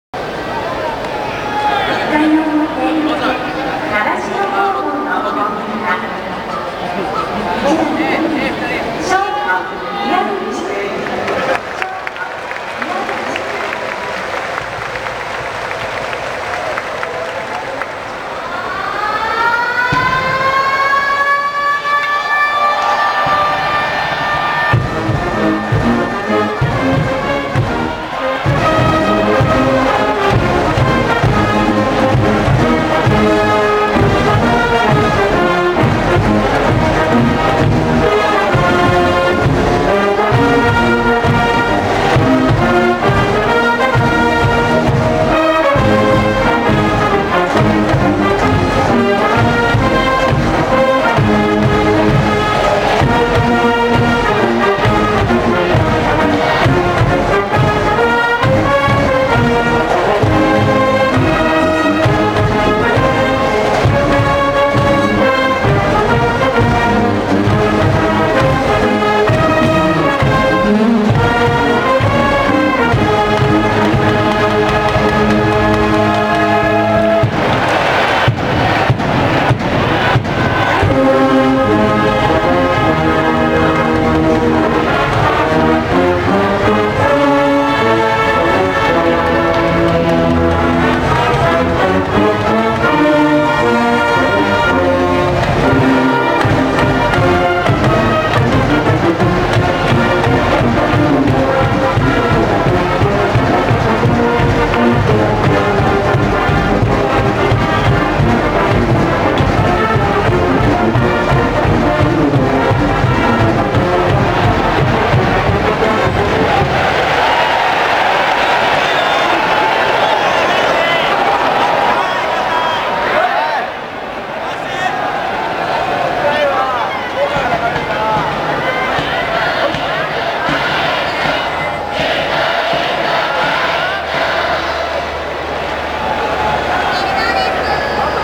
静岡－習志野 １回表
２０１１年８月７日
第９３回全国高等学校野球選手権大会　１回戦
静岡　－　習志野

習志野の３塁側アルプスからの撮影です。